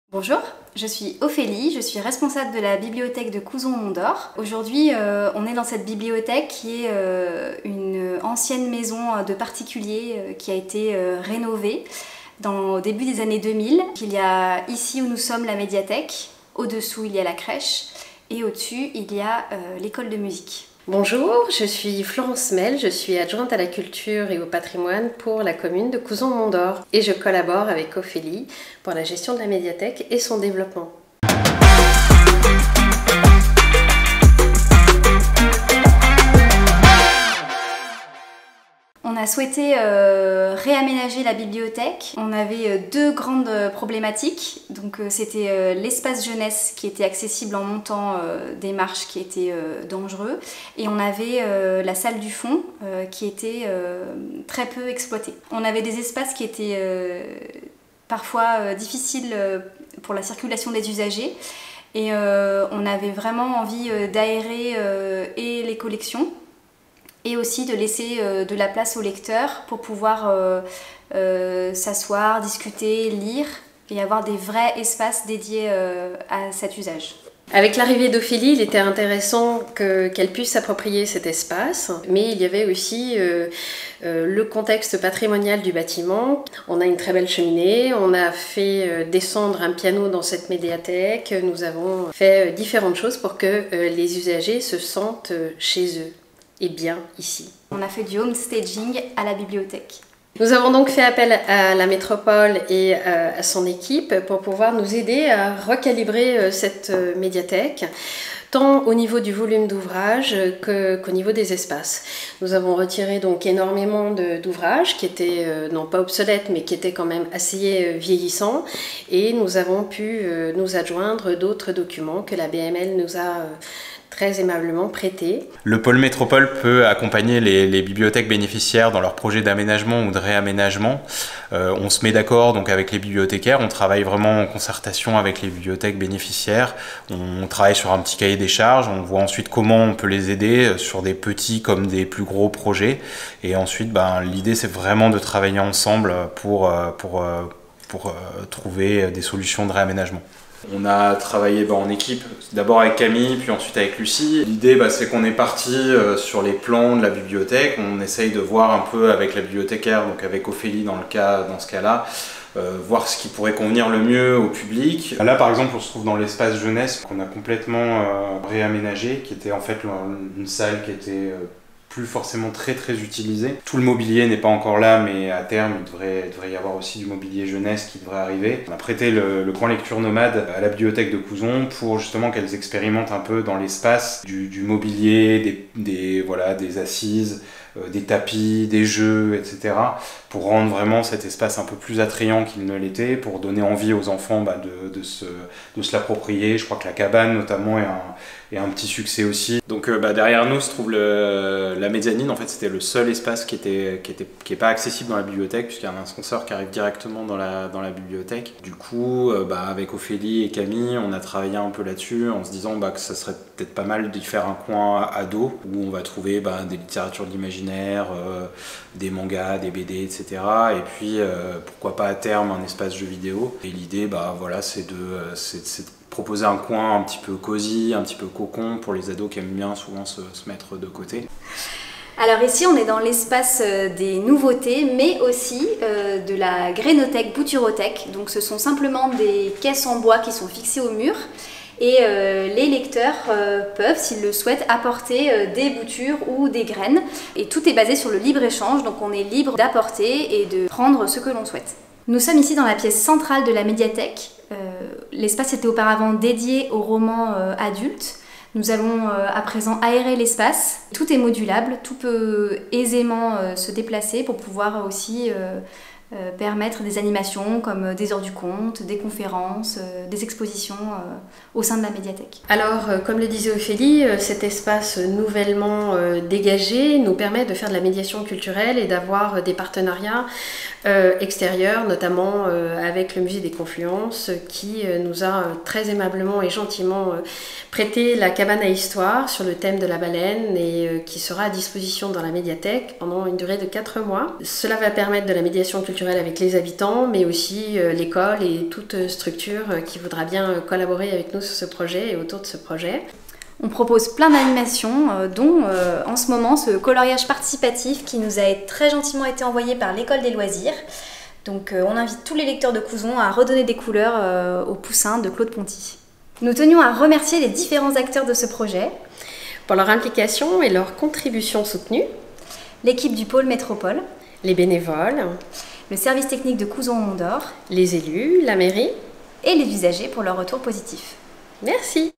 Du home staging à la bibliothèque !
Quoi ? Du home staging à la bibliothèque ?! Ophélie, Florence et Nicolas présentent un projet de réaménagement à la Médiathèque de Couzon au Mont d’Or avec la Métropole de Lyon, qui a repris quelques préceptes du homestaging. Dans ce reportage, il est aussi question de livres prêtés par la Bibliothèque municipale de Lyon, d’un partenariat avec le Musée des Confluences et d’un coloriage participatif de l’école des loisirs avec le poussin de Claude Ponti.

Vidéo sous-titrée en activant l'option concernée.

Réalisation : Thomas Iglésis